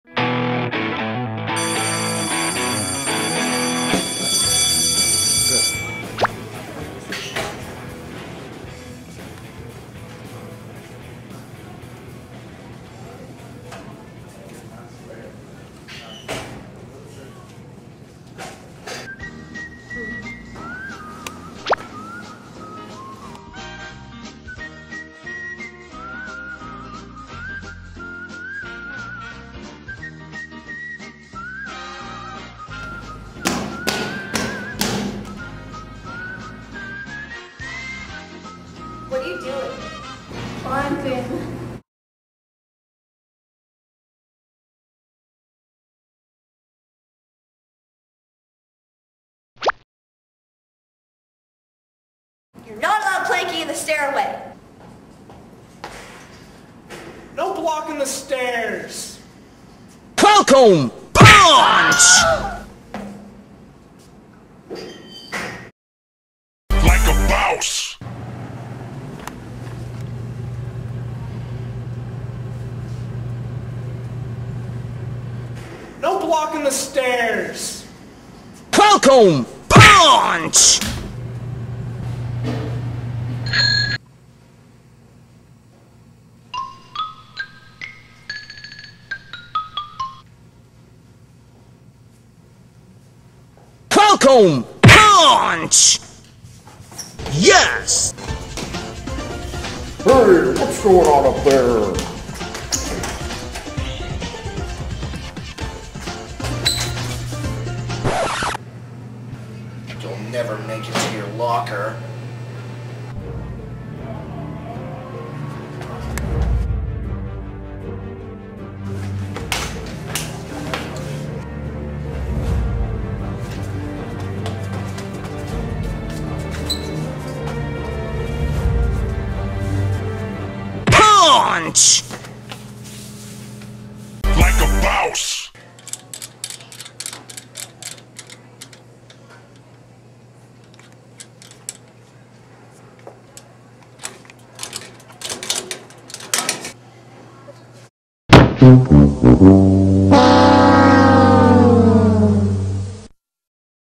Logan Van Spronsen vs. The School (School Project)
Logan just wants to get to his locker, but Kayla, Vanessa and James are mad at him (for whatever reason) and wont let him get to his locker that easily.